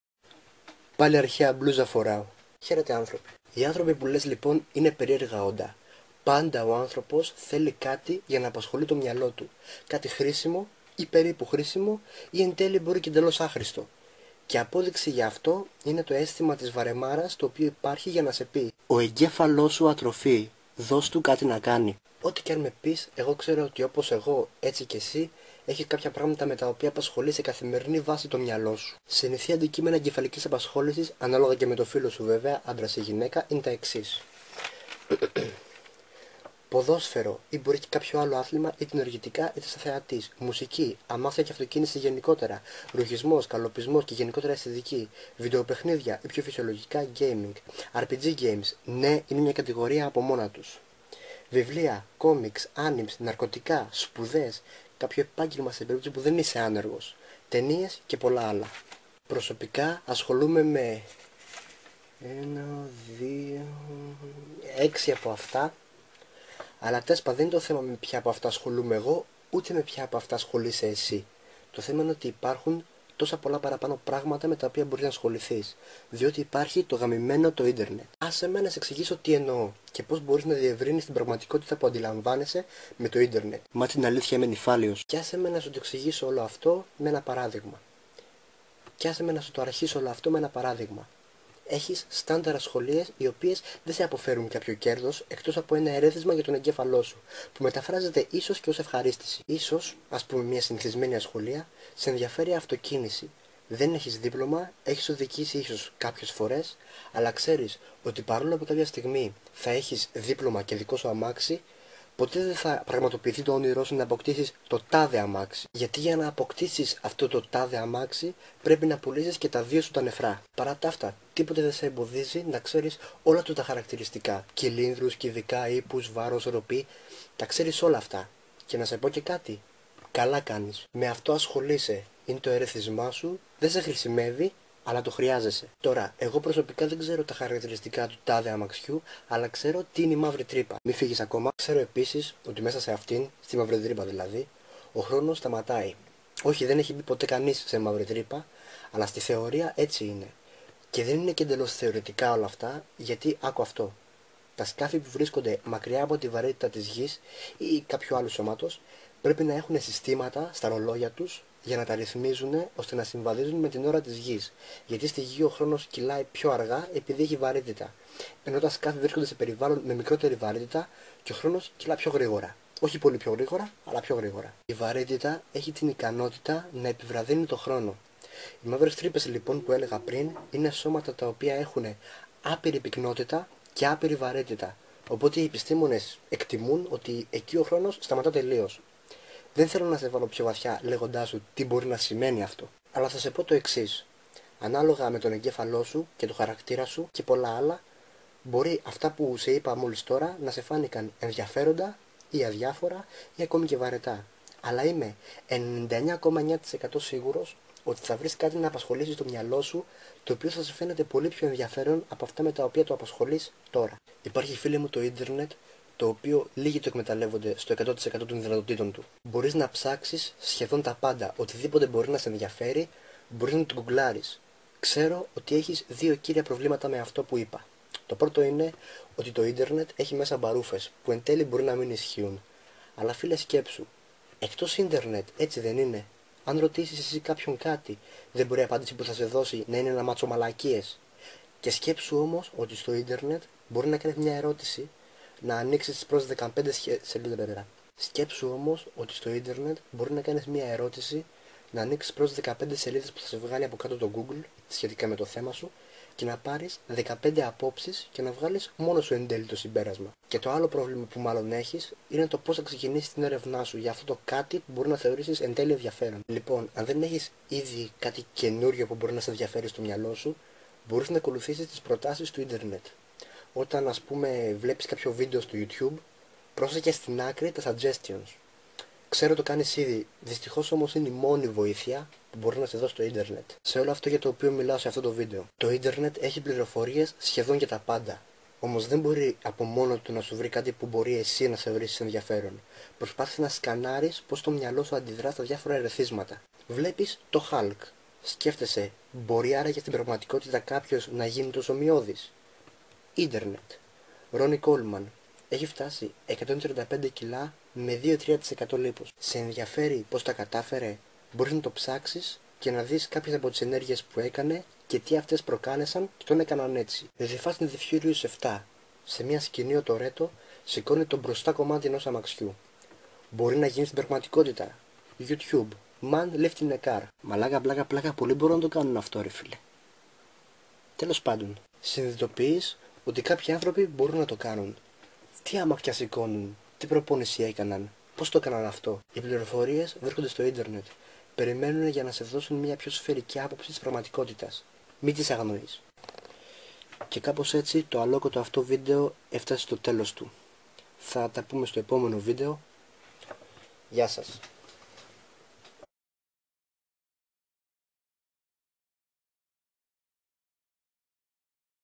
Ο κόσμος είναι μεγαλύτερος από όσο νομίζουμε
δεν είναι και τόσο "κομικό" το βίντεο όπως είναι τα υπόλοιπα μη workout videα του καναλιού αν και έβαλα λίγα αστεία μέσα ώστε να μην είναι βαρετό μέχρι αυτοκτονίας..αυτό το βίντεο έχει σκοπό να σε εξηγήσει πως μπορείς να ξεφύγεις λίγο από τα συνιθησμένα σου θεωριτικά ενδιαφέροντα και να ανακαλύψεις σε θεωριτικό επίπεδο πτυχές του κόσμου που θα σε φανούν πιο ενδιαφέροντες από τα ενδιαφέροντά σου..όλα αυτά με την βοήθεια του ίντερνετ..